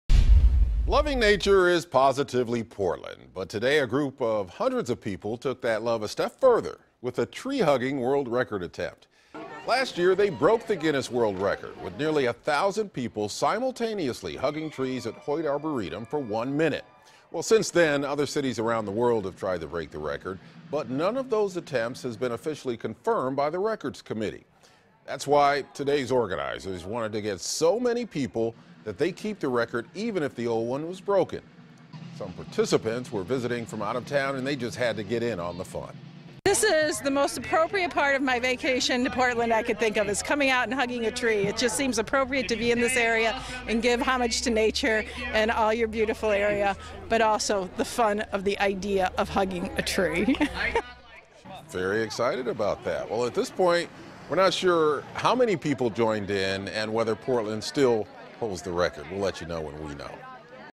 Portland tries to set new tree hugging record
Portland tried to break its own record for most people hugging trees simultaneously on Saturday.